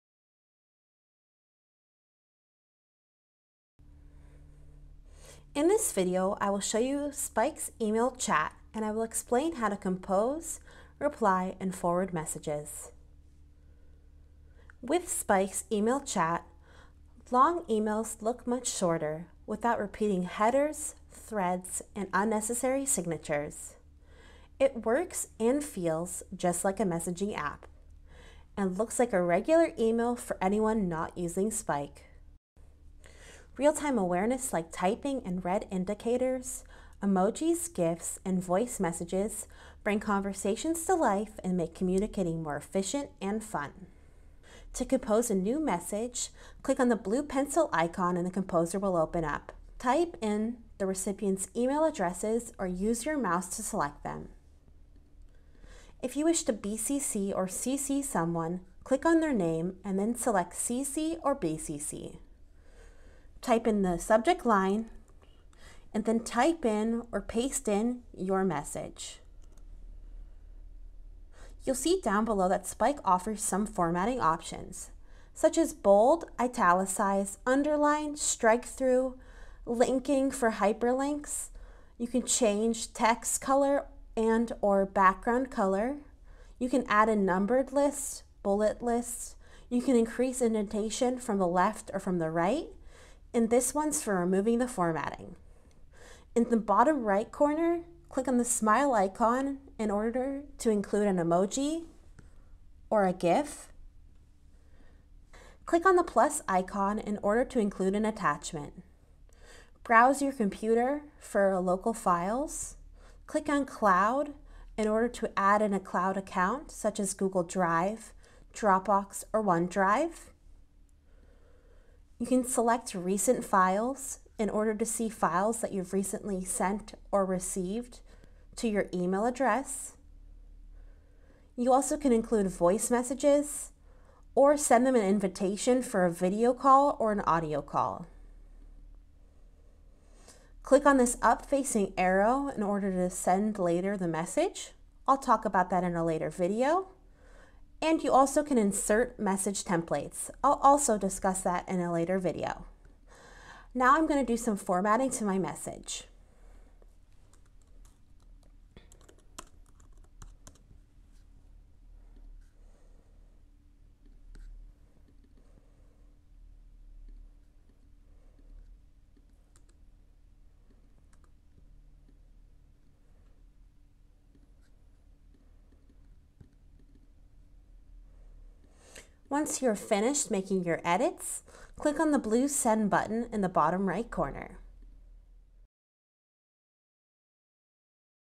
Spike How-to: New Message
Learn how to send a message on the Spike desktop or web app.

for more info on Spike’s superpowers.